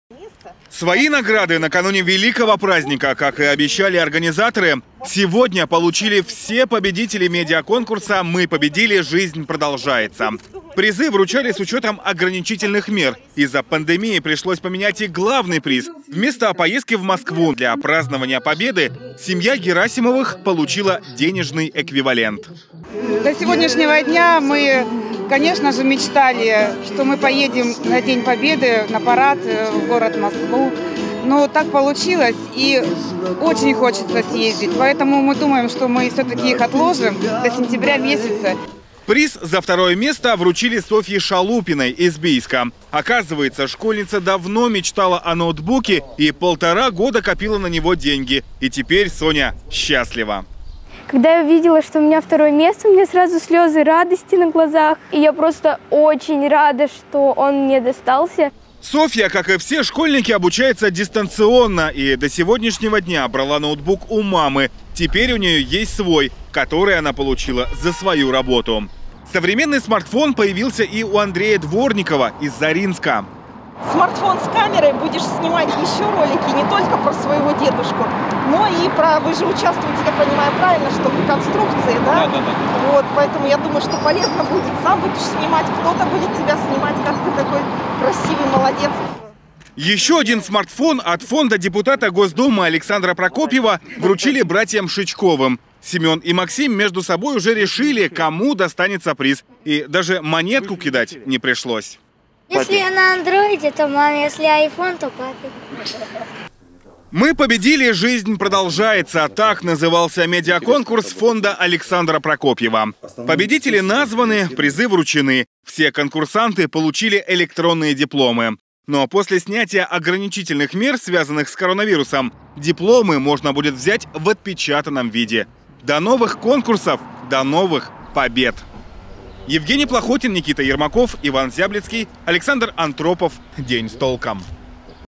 Победителям конкурса видеороликов «Мы победили. Жизнь продолжается» вручили ценные призы
Сегодня в Алтайском крае вручили призы – победителям медиа-конкурса, посвященного 75-летию Великой Победы. Проект «Мы победили. Жизнь продолжается» организовал фонд депутата государственной Думы Александра Прокопьева. Изначально планировалось торжественное награждение. Но из-за пандемии – пришлось отказаться от публичного мероприятия.